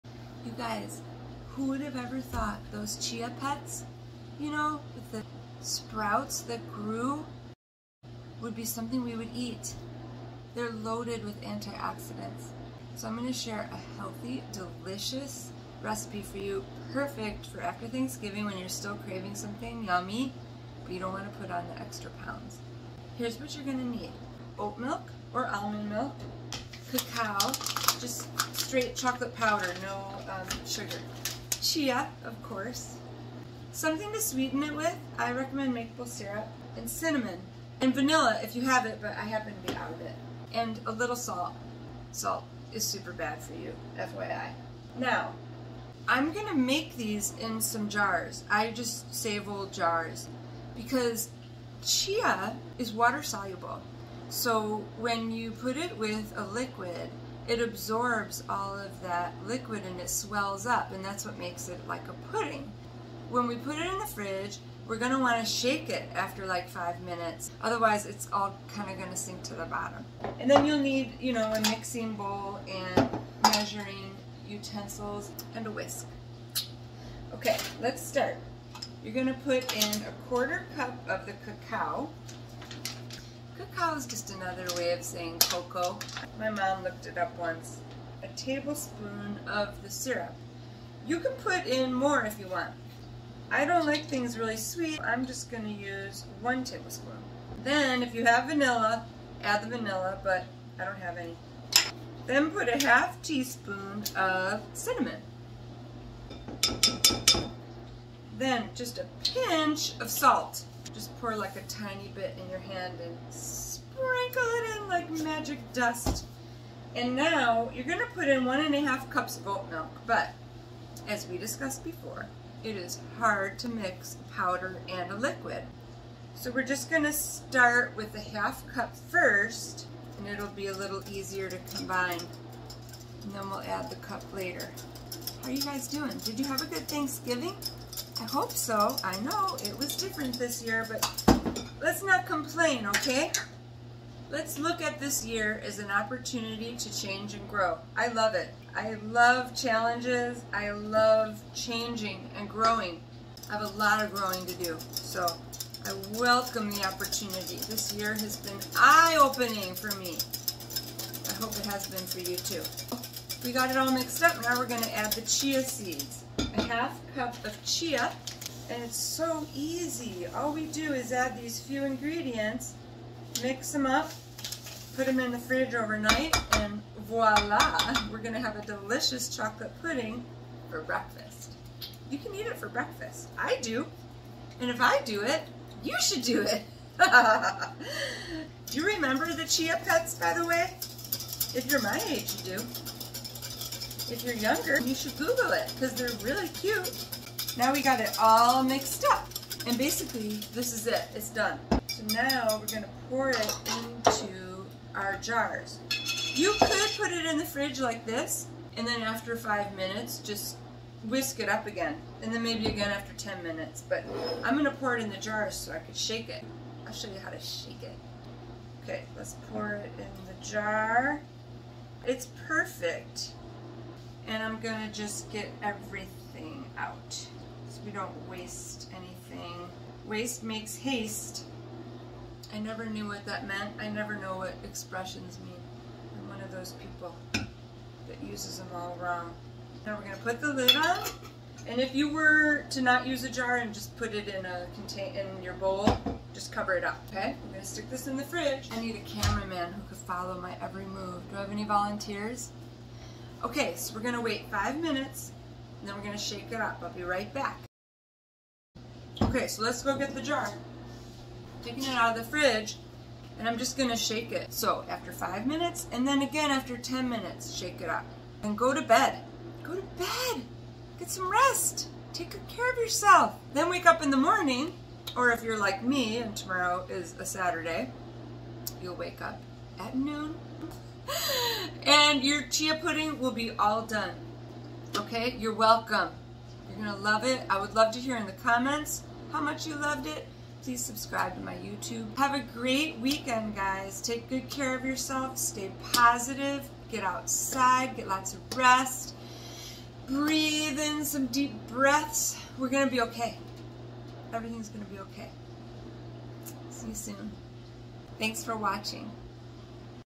How to Make Vegan Chocolate Chia Pudding!
Your Homespun Honey is back with one of my favorite desserts! iIt's easy, delicious and good for you!

Chia is full of antioxidants, fiber, protein and Omega 3 fatty acids. They were a main staple of the Aztecs.

Why are antioxidants good for you? Oxidation produces free radicals which can lead to cancer snd heart disease. So we want to eat foods that inhibit that.

And Omega 3 fatty acids are good for your heart!